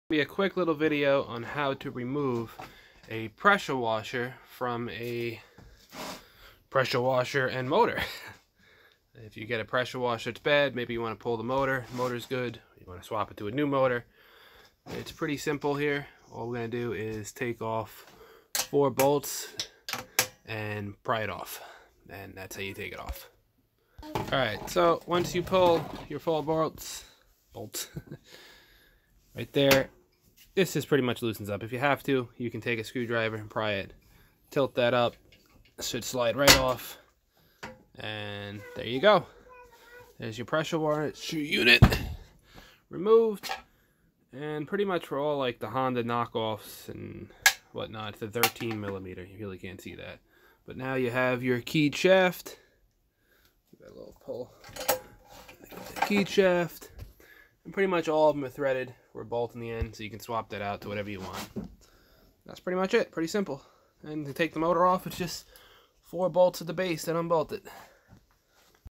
Quick removal of pressure washer pump . GX/ Predator Honda Clone
pretty much the same for all predator,  Simpson  and honda pressure washers . great motors for go carts or mini bikes.